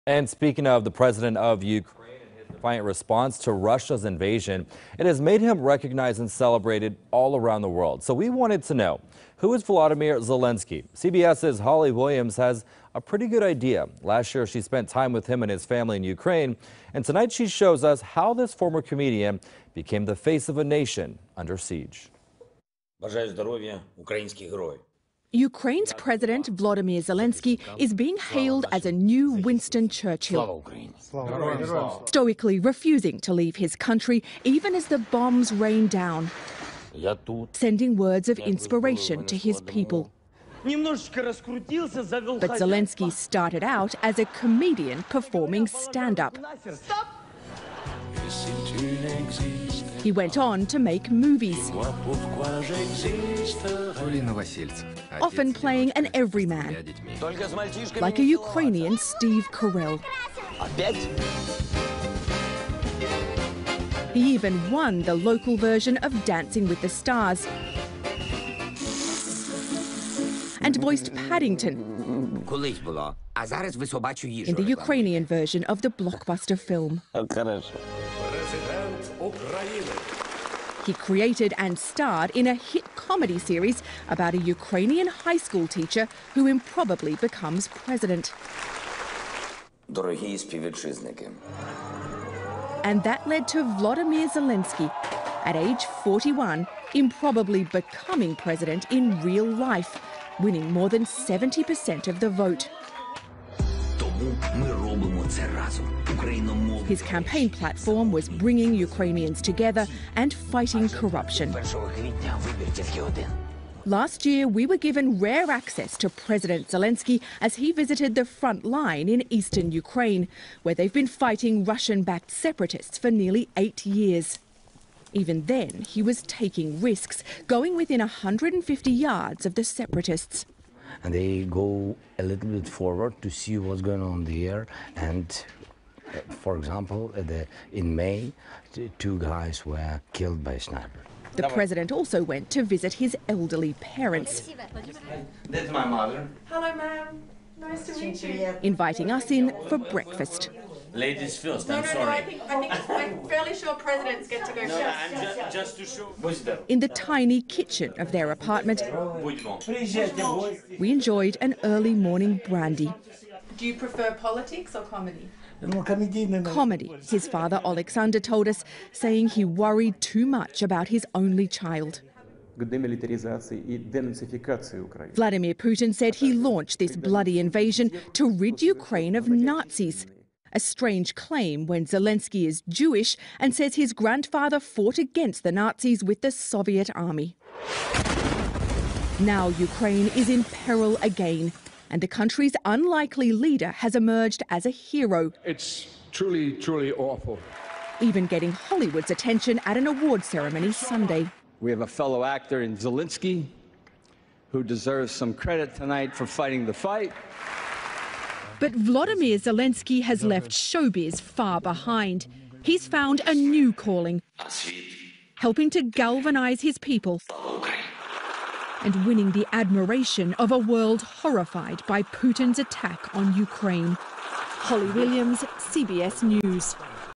Who is Volodymyr Zelenskyy? CBS News Provides a Background on Ukrainian President
Volodymyr Zelenskyy has earned the hearts of millions around the globe as he continues to stand for peace in Ukraine, amid the Russian Invasion, but who is the Ukrainian President? CBS News has his background, which began in a more surprising place than you might think.